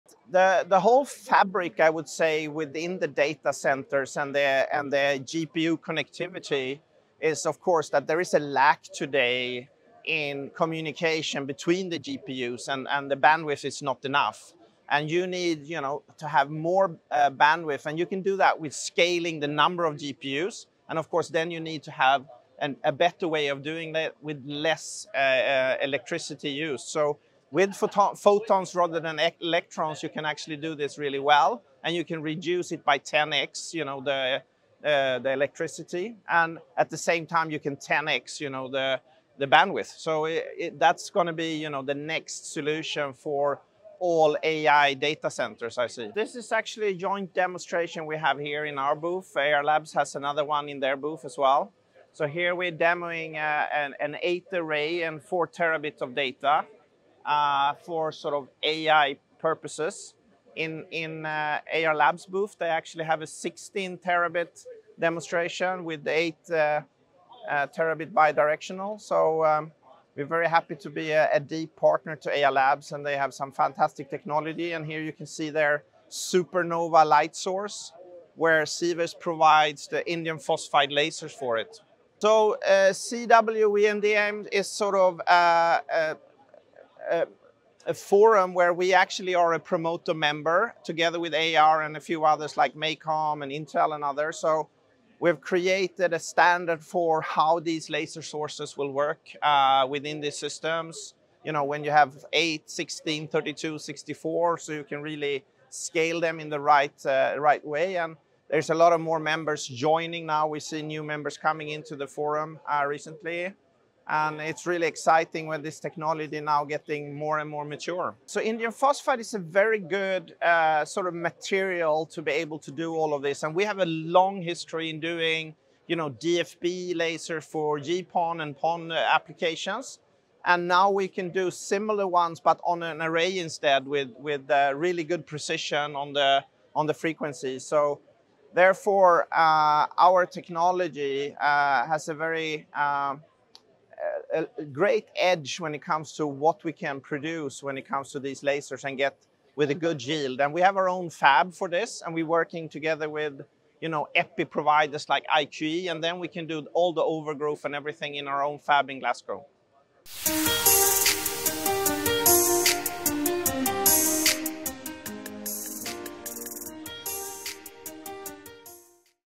OFC24: Advanced DFB Laser Arrays for I/O in AI Clusters
Sivers Photonics is supplying its DFB laser arrays for Ayar Labs’ SuperNova optical interconnect for AI/ML clusters, disaggregated data centers, 6G networks, phased array sensor systems and etc. Anders Storm, CEO from Sivers Semiconductors explains:
- The current lack of communication and insufficient bandwidth between GPUs can be addressed by scaling the number of GPUs and using photons instead of electrons. This approach not only increases the bandwidth by 10x but also reduces electricity usage by the same factor.
- Sivers Semiconductors is partnering with Ayar Labs to demonstrate an eight-array optical interconnects capable of 4 Tbps, scaling to much higher rates
- The company is a promoter member of CW WDM, a forum that has created a standard for how laser sources will work within these systems. The technology is maturing, with new members joining the forum and the use of Indian phosphide lasers proving to be a very effective material for these applications.